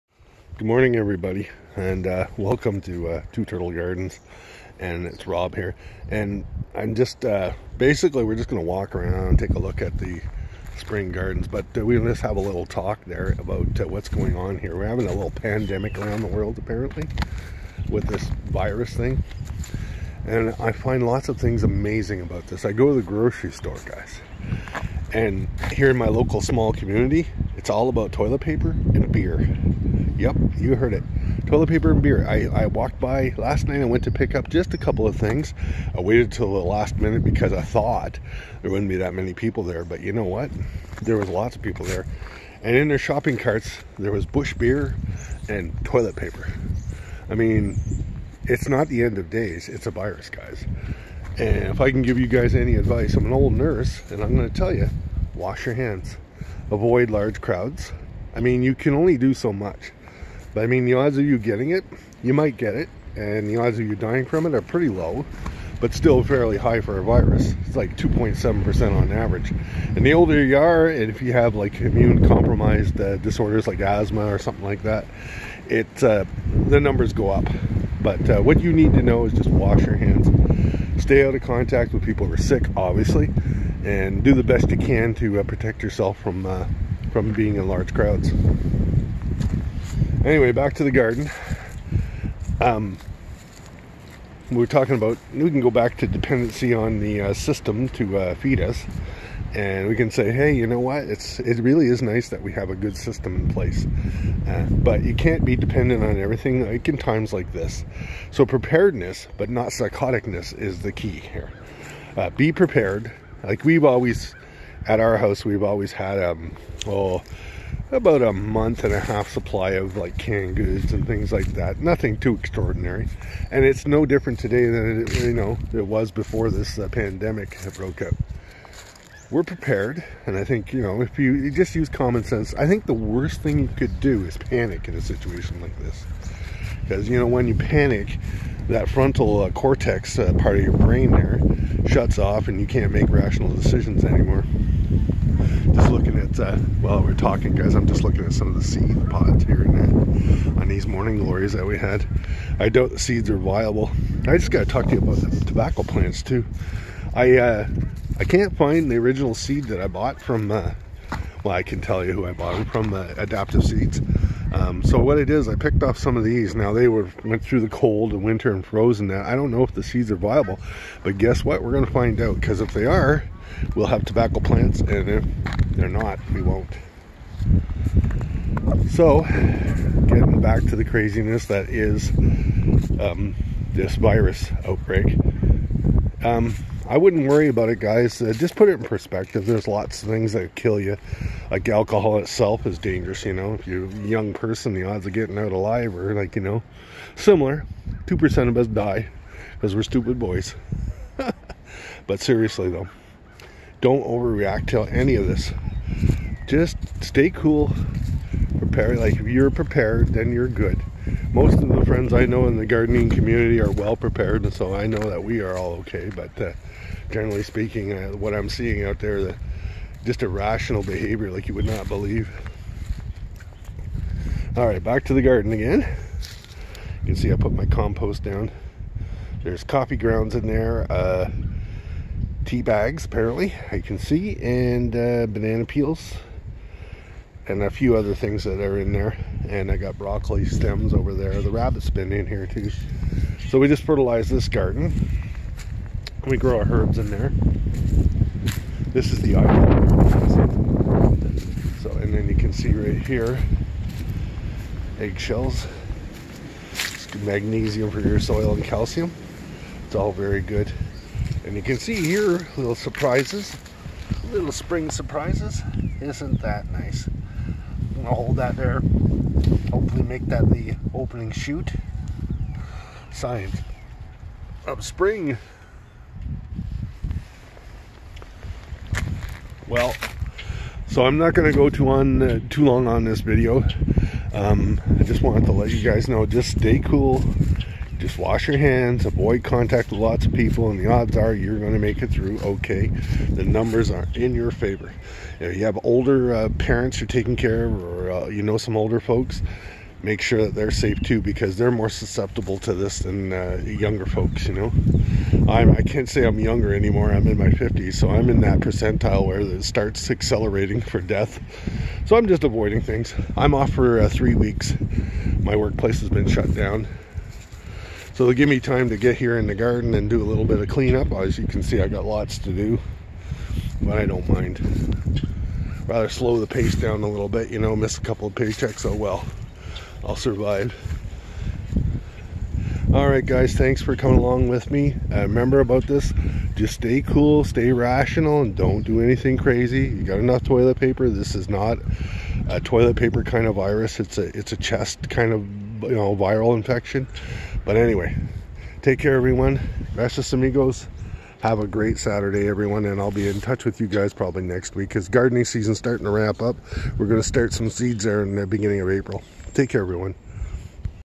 Spring Tour /Irrational panic and Quarantine
Today I rambling on about irrational behavior around the pandemic.  I think some folks are in full panic mode and I think that is not useful.